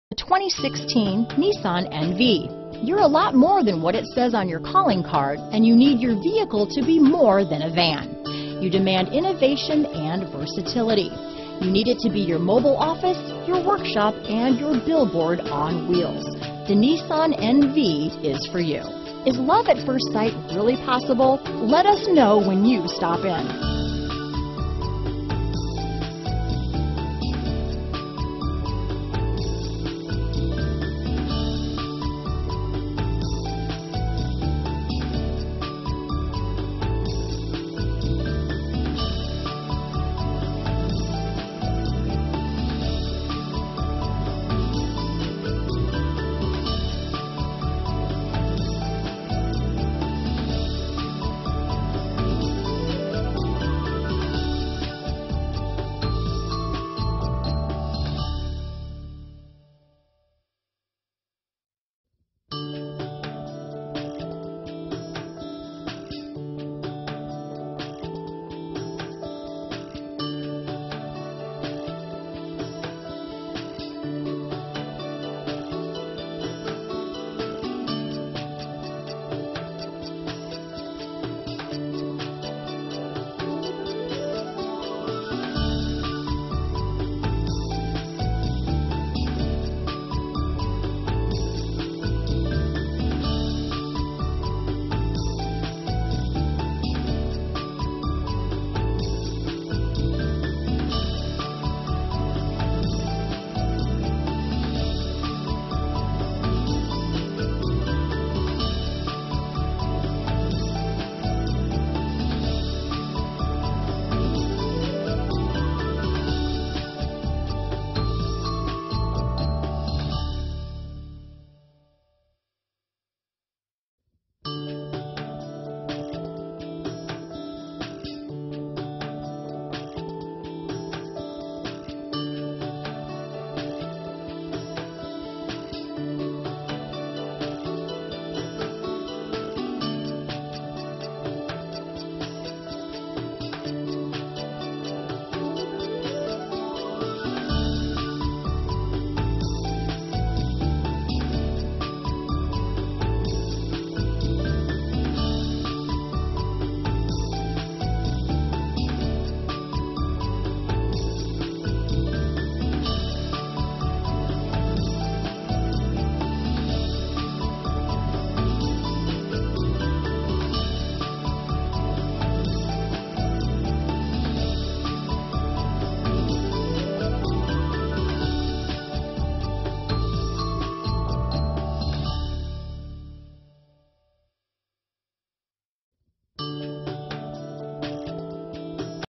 2016 Nissan NV Clinton, Fayetteville, Goldsboro, Raleigh, Elizabethtown, NC 3386
2016 Nissan NV

Radio: AM/FM/1CD/MP3 Audio System w/4-Speakers -inc: RDS  auxiliary input and steering wheel audio controls,4-Wheel Disc Brakes w/4-Wheel ABS  Front And Rear Vented Discs and Brake Assist,Tire Specific Low Tire Pressure Warning